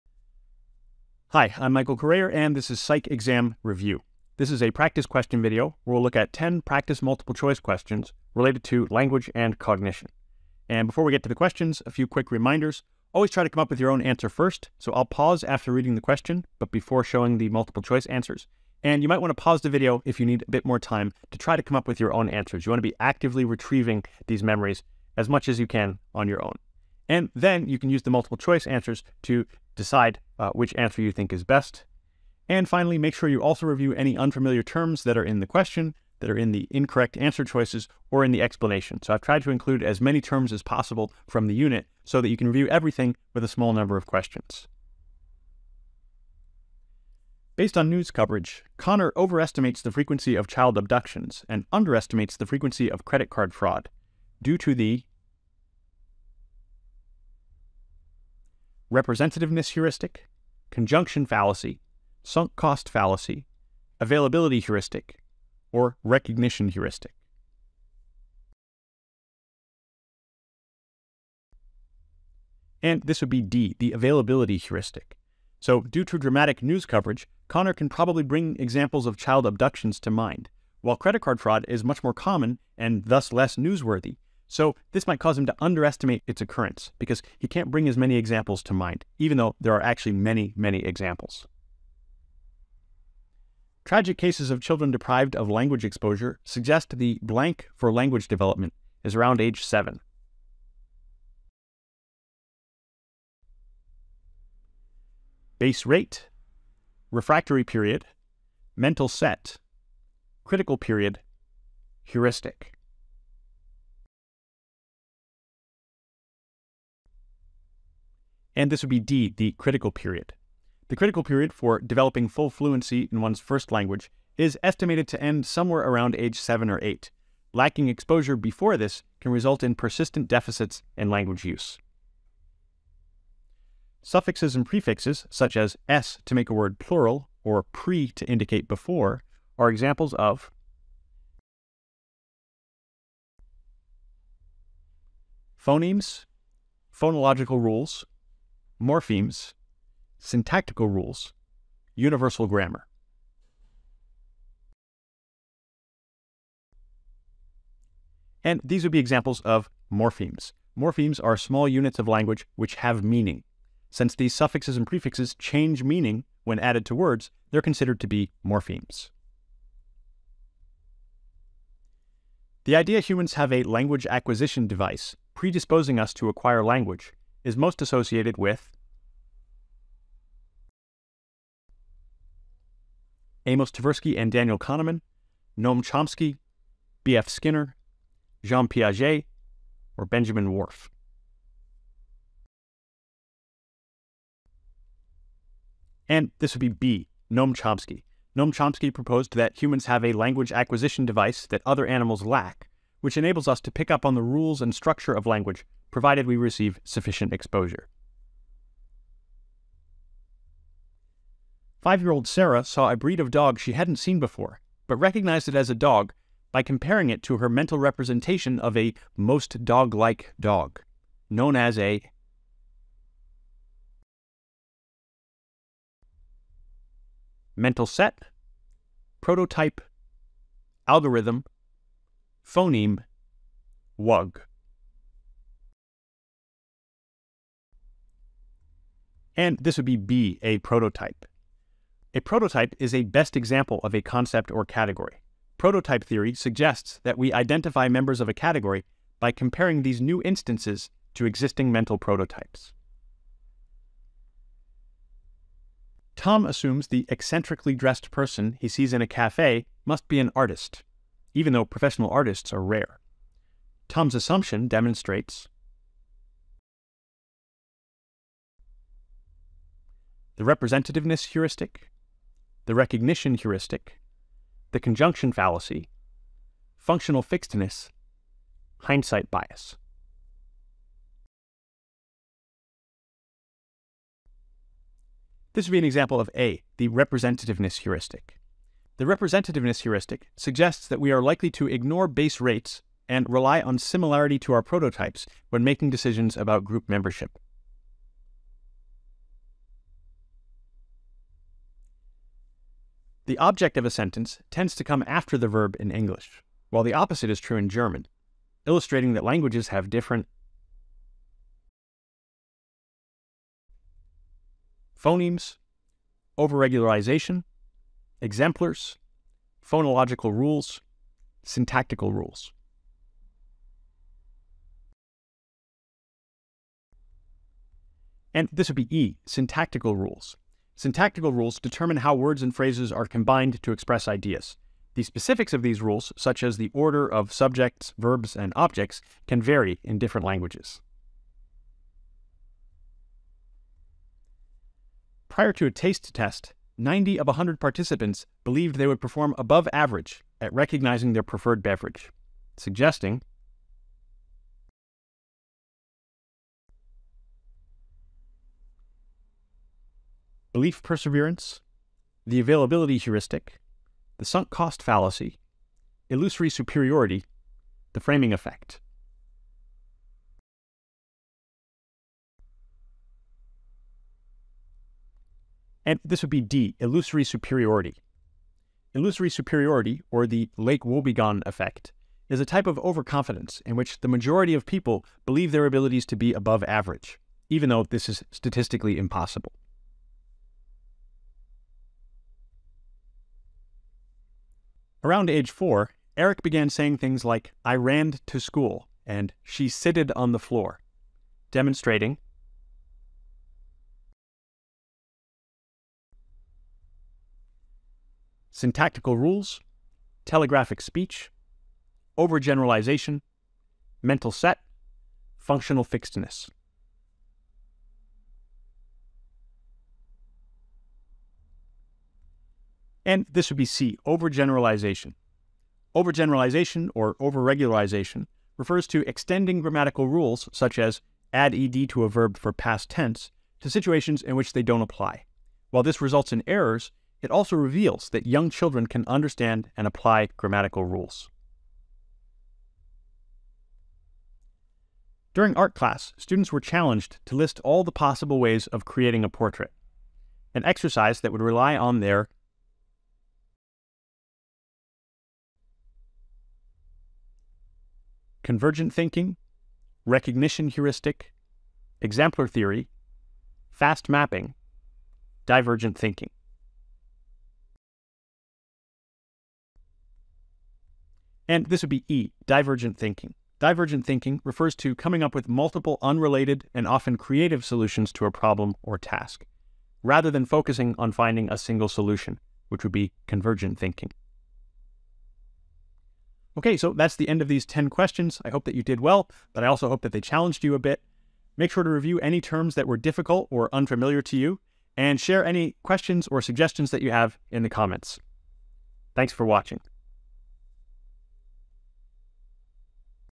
Psychology Review Questions - Language & Cognition
This video covers 10 multiple choice practice questions on language & cognition and includes concepts related to linguistics, language acquisition, problem-solving, decision-making, heuristics, and more. I hope this can help you to review your knowledge and understanding, let me know if you have any suggestions for future practice and review videos!